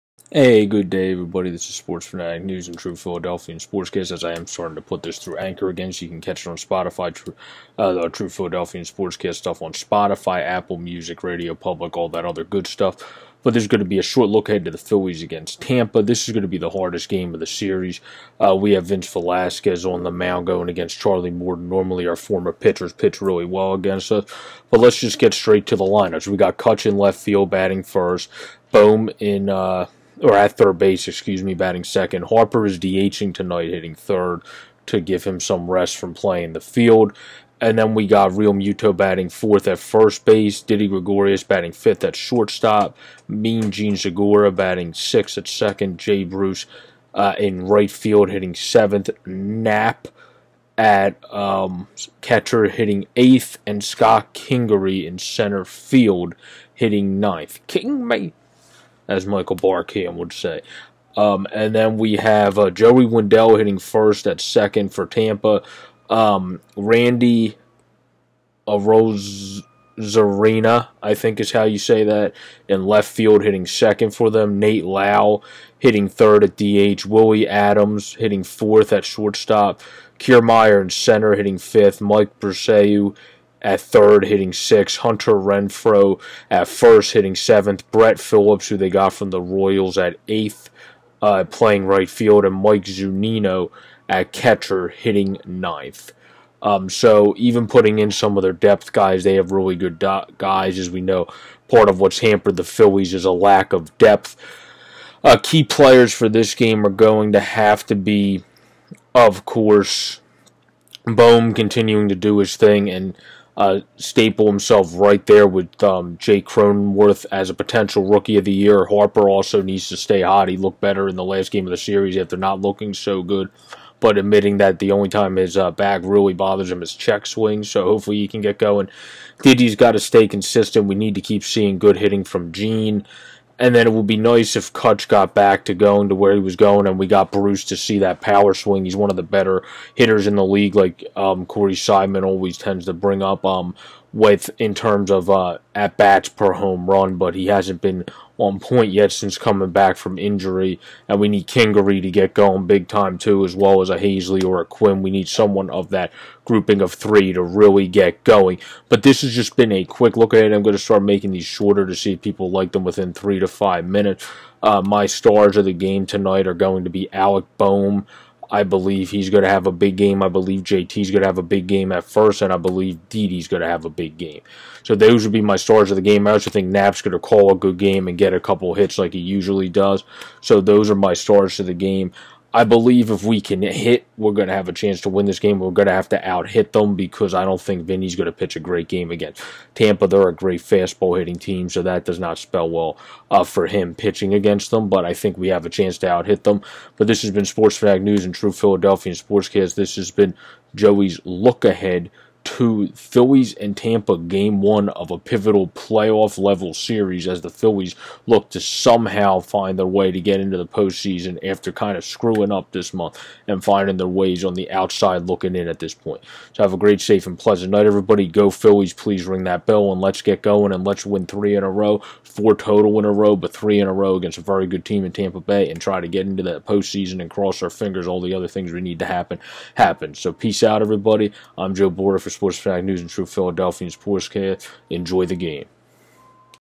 Phillies Vs Rays Pregame
Quick Pregame to Phillies and rays where I break down key pieces in the lineups plus the pitching match up. We need to win this one and need Vinny to at least pitch decent.